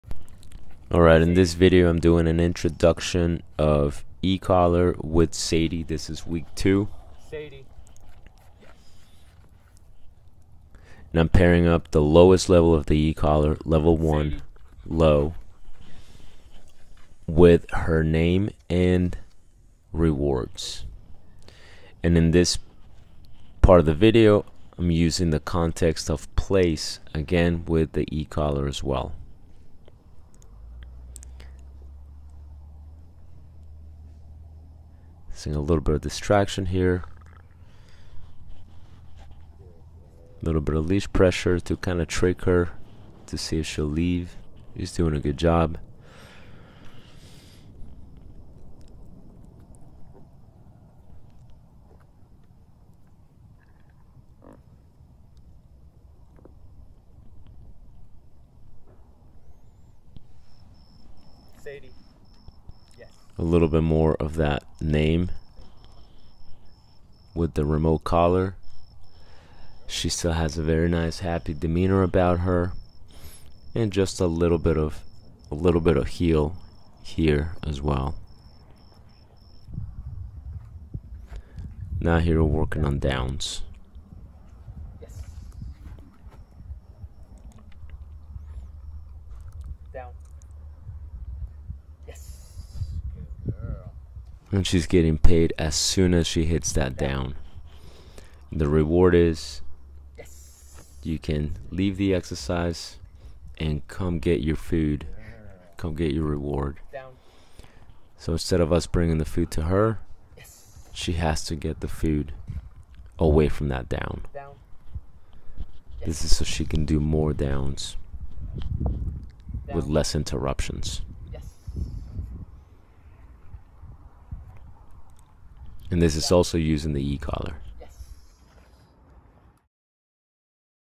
Great Pyrenees E-Collar Intro. Sadie, Week 2
Sadie getting introduced to the E-collar.

For my protection book, check out: